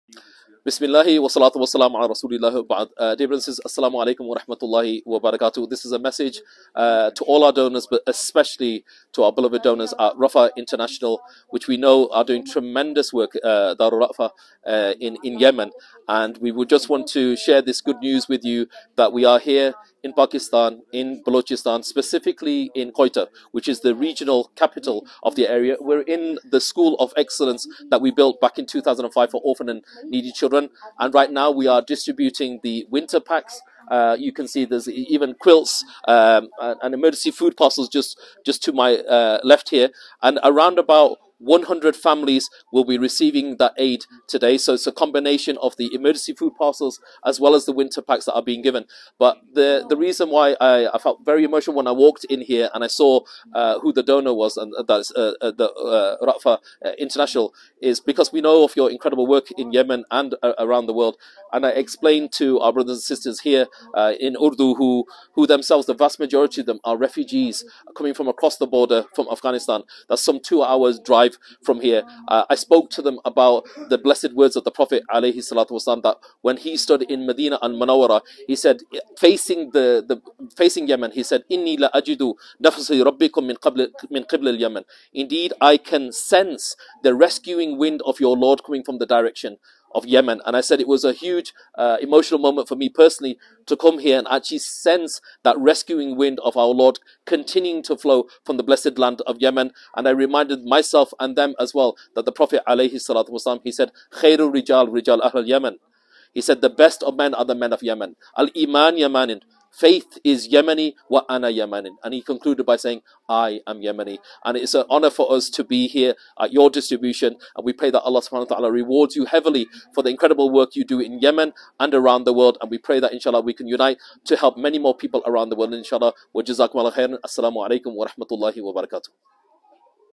Pakistan Floods Update with Muslim Hands
An update on our ongoing Pakistan Flood Appeal campaign from our amazing brothers and sisters @muslimhands who are currently distributing the aid in some of the most heavily affected areas. May Allah reward them for their efforts and support.

To contribute to the ongoing emergency campaign in Pakistan please